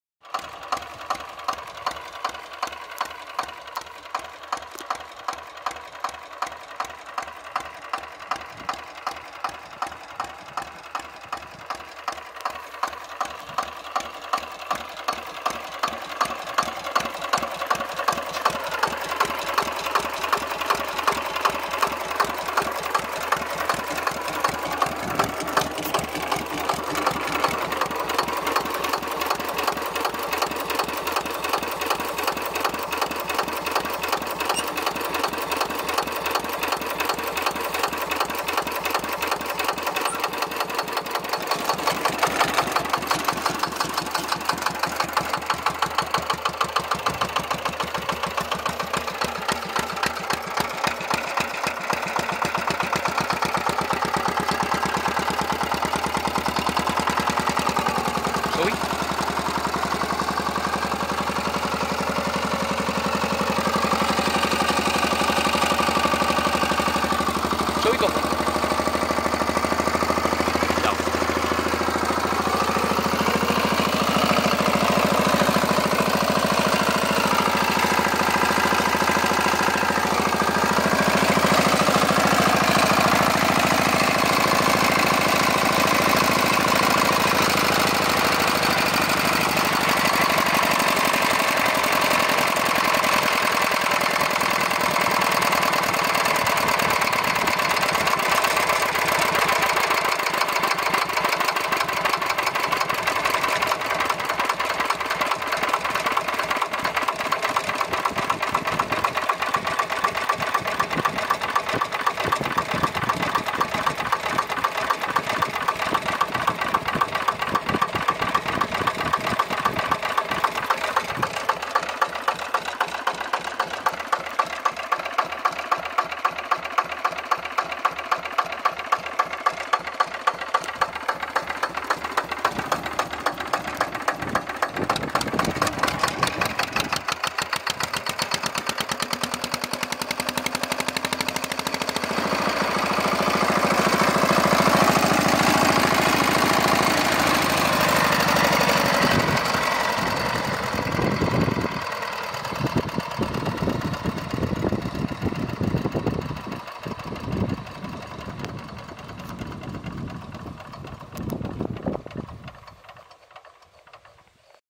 Walk-around eigenbouw Lister Petter paz 1 Diesel Wheelhorse tractor
Rondje om mijn eigenbouw creatie die laat zien hoe het allemaal in elkaar steekt

Mini lanz buldog 😊